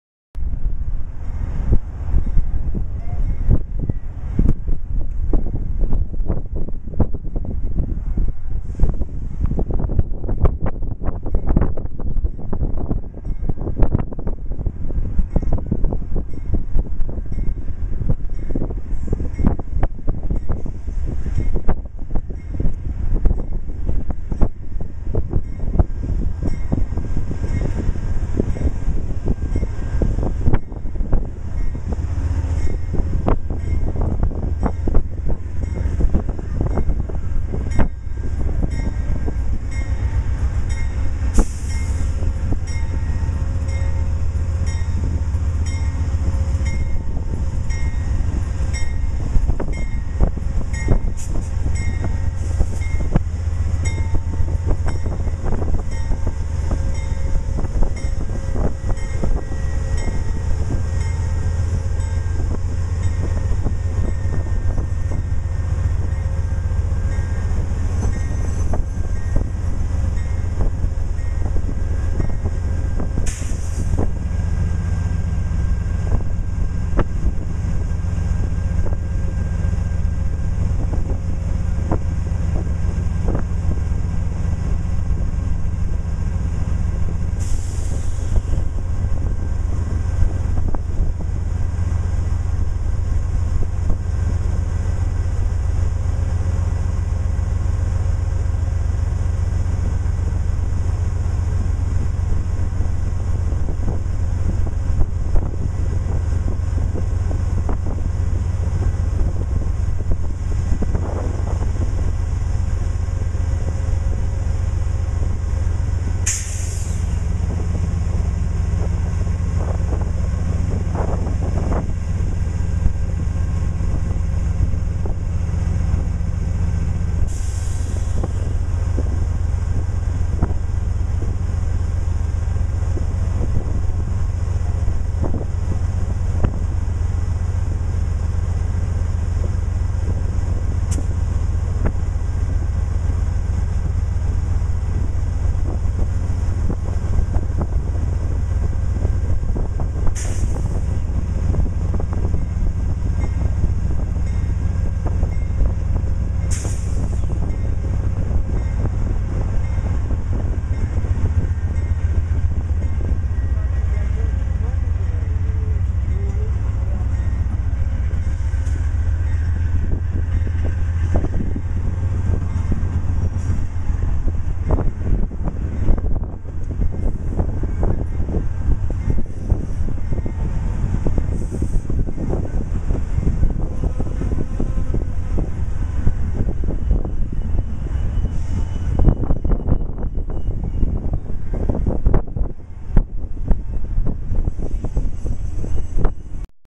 Amtrak Texas Eagle arrives at Joliet
Amtrak P42DC #83 leads the TE into JUS.